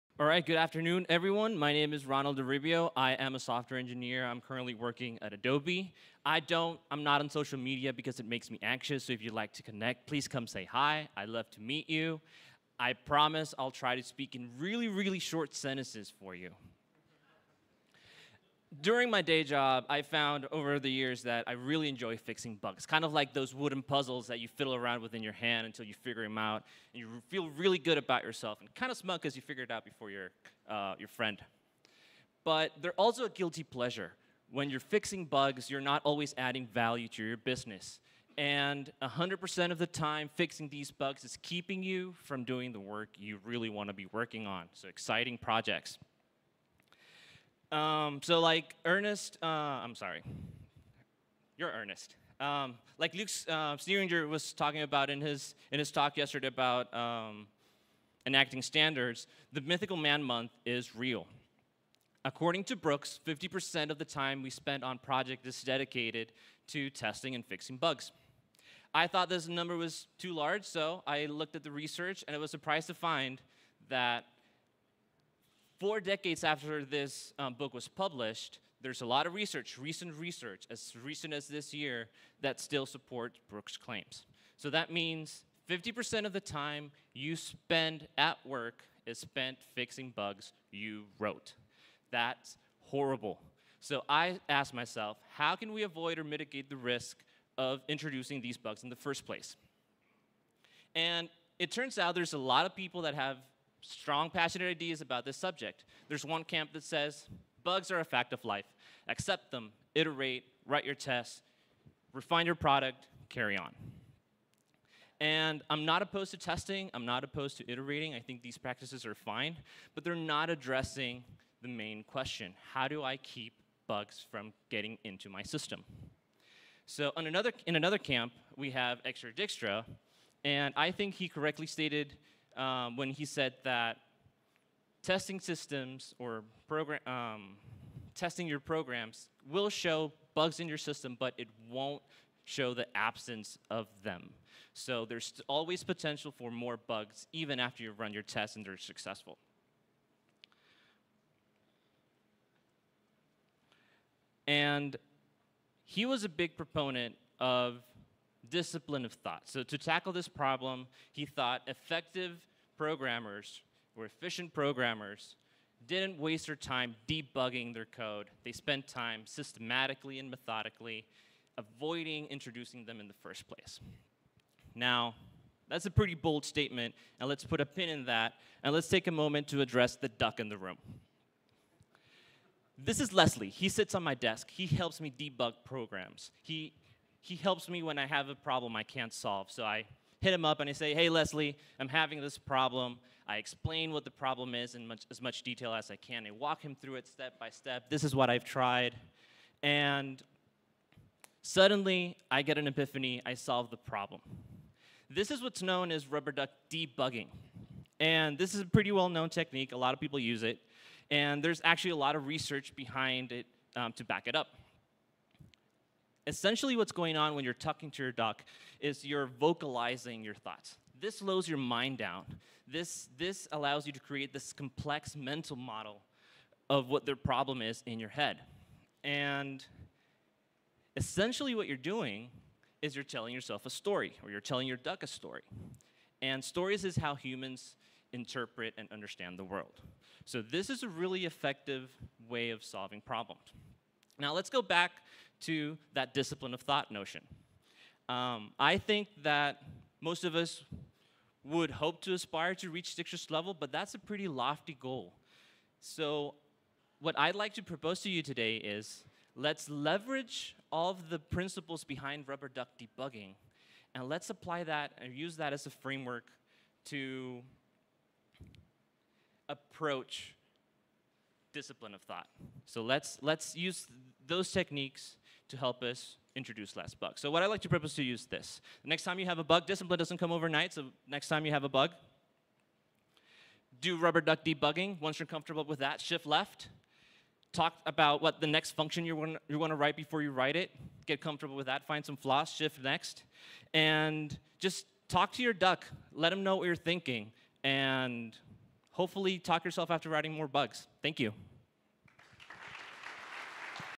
Ignite! Ronald E. Oribio R. - See the forest through the trees
DevOps Days Austin 2019 Day 2